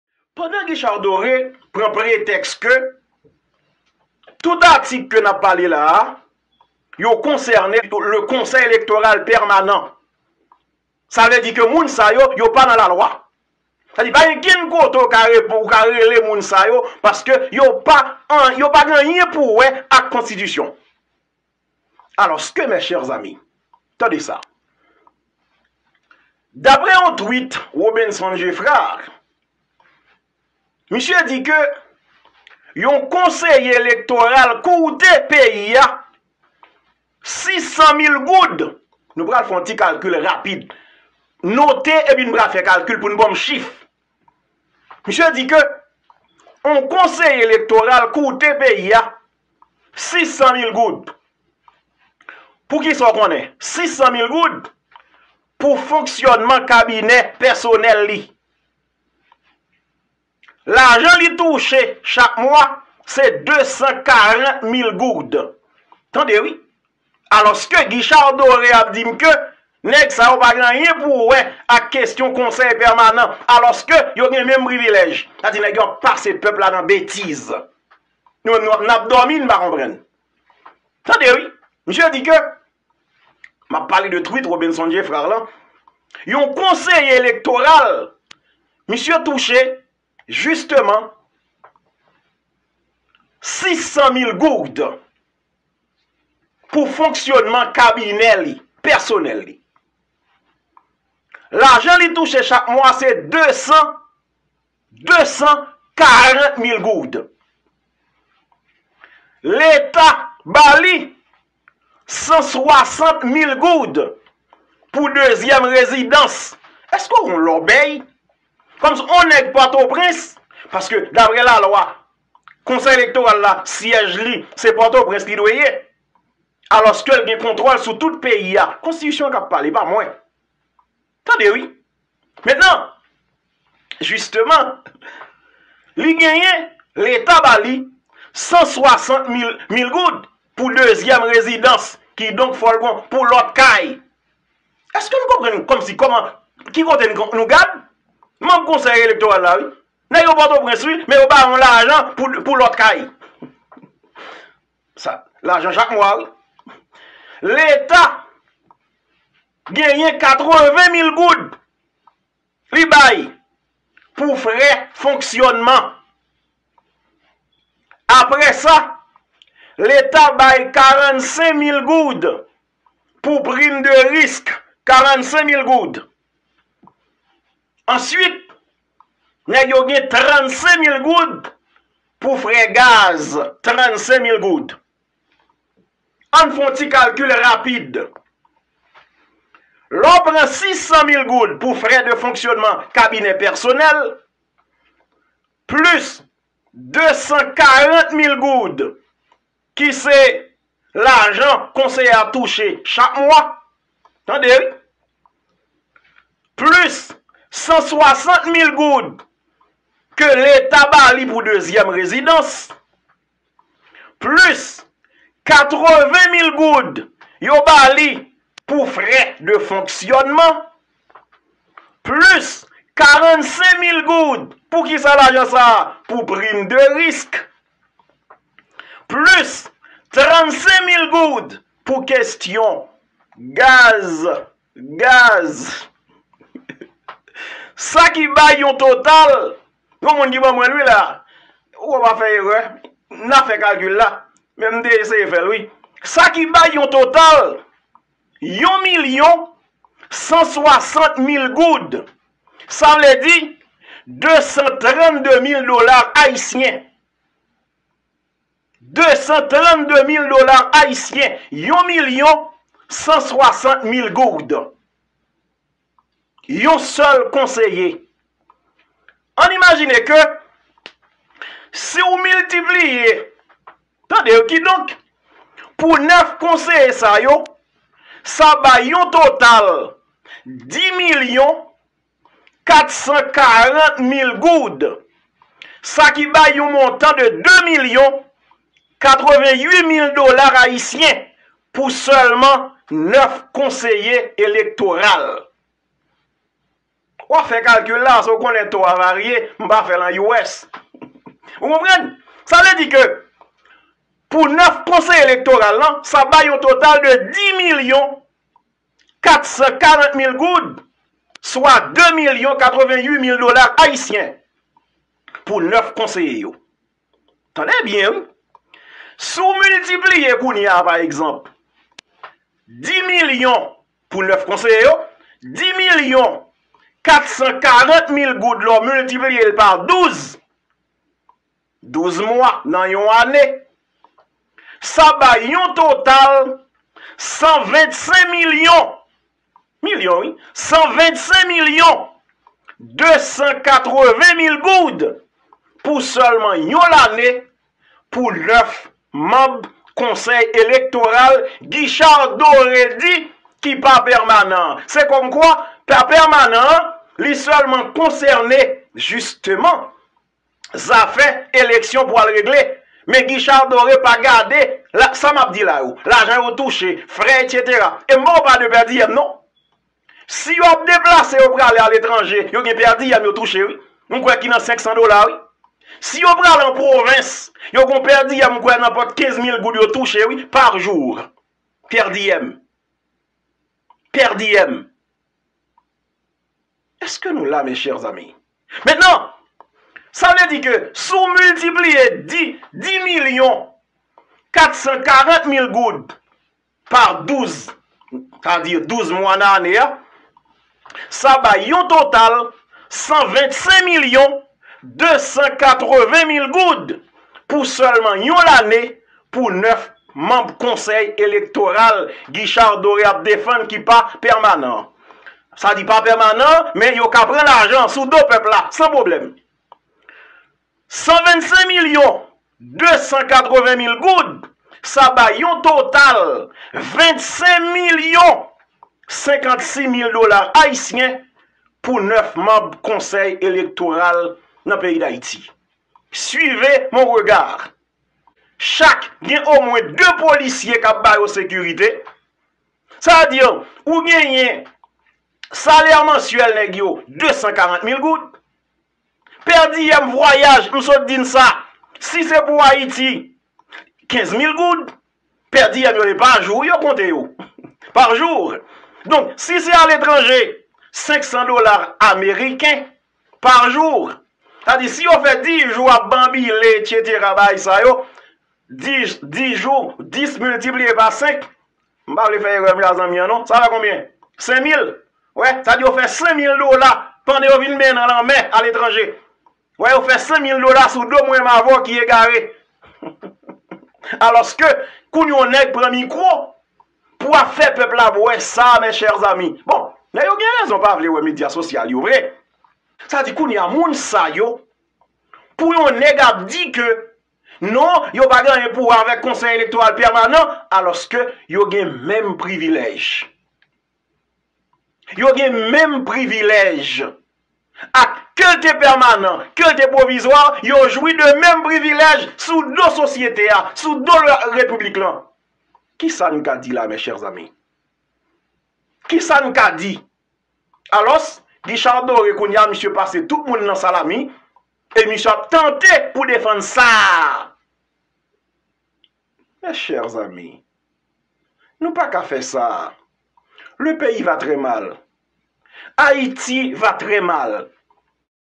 Yon ide sou kantite lajan yon manm KEP ap touche pandan Guichard Doré ap pase sosyete a nan rizib.
Yon manm Konsèy Elektoral Pwovizwa gen menm privilèj ak yon manm konsèy Pèmanan Kidonk yo tout dwe obeyi anba konstitisyon an.